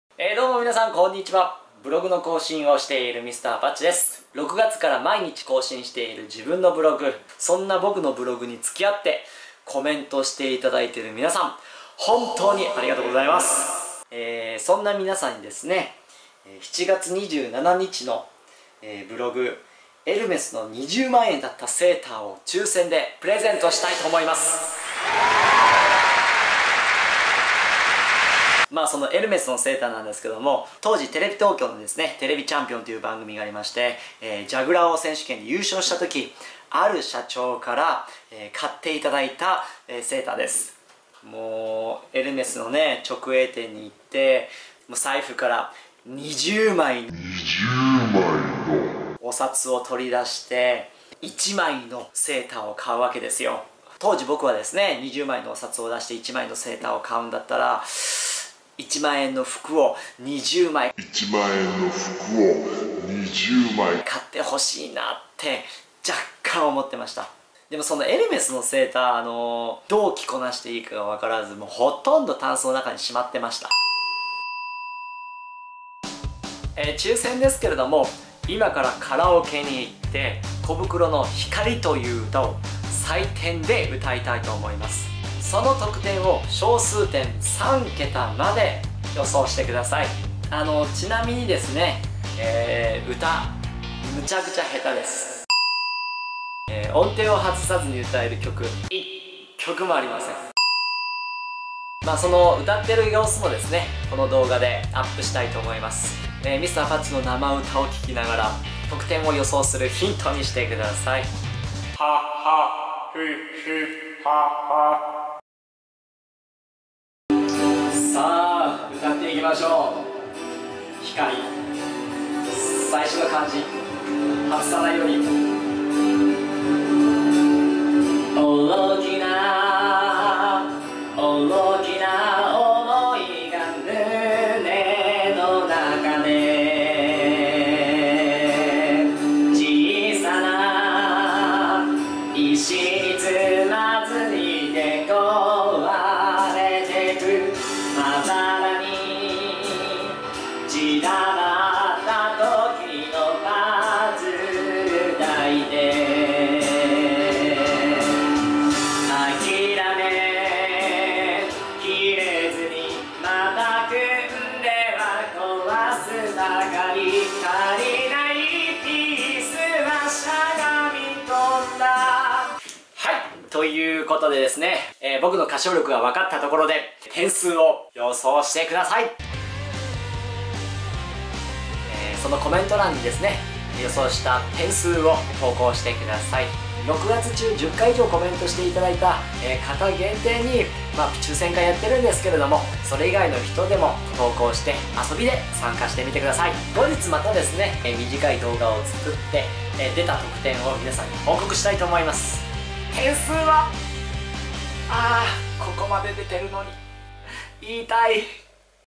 ２０万円だったエルメスのセータープレゼントはカラオケ採点で～MrapatchTV
Mr.アパッチブログコメントして頂いてる方へのプレゼント企画
２０万円だったエルメスのセーターをプレゼント企画動画。

撮影・編集・出演/Ｍｒ．アパッチ

Please click and watch YouTube advertising.